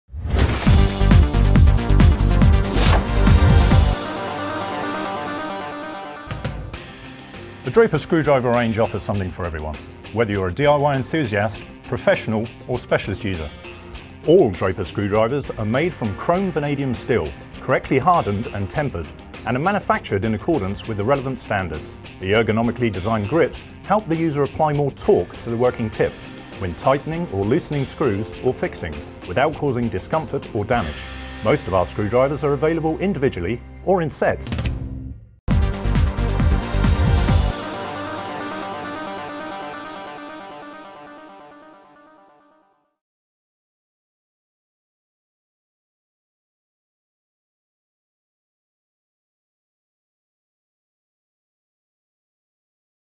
Draper Screwdriver Range From MicksGarage.com
The MicksGarage.com Experts demonstrates Draper Screwdriver Range available to buy